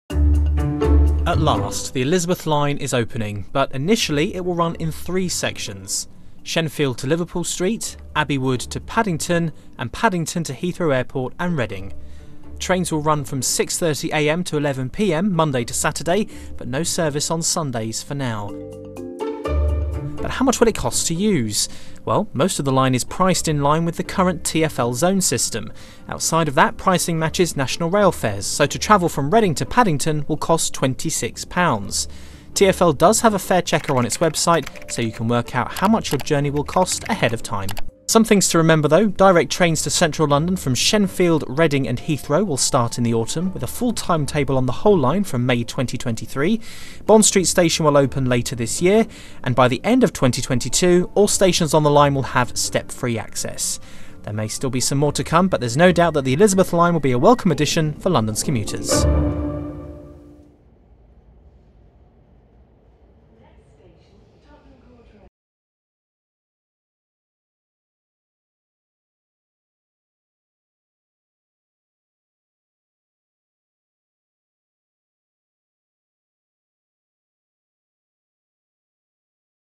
Elizabeth line: explained
The Elizabeth line opens officially at 06:30am on Tuesday 24 May, but what can commuters expect when using the service? BBC London's Paul Murphy-Kasp has put together this guide.